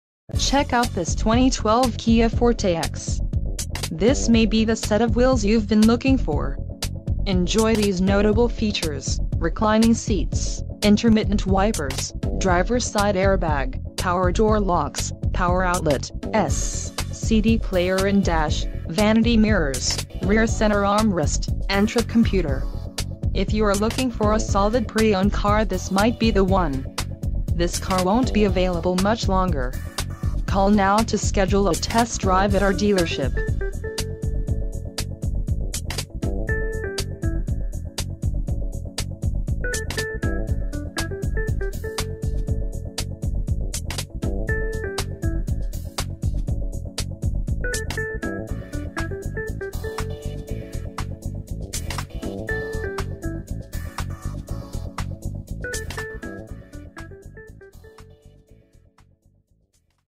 2012 Kia Forte - Ivory Chevrolet - Union City, GA 30291
Check out this 2012 Kia Forte EX. This may be the set of wheels you've been looking for. Enjoy these notable features: RECLINING SEATS, Intermittent Wipers, DRIVER SIDE AIR BAG, Power Door Locks, Power Outlet(s), CD Player in Dash, Vanity Mirrors, Rear Center Armrest, and Trip Computer. If you are looking for a solid pre-owned car this might be the one. This car won't be available much longer.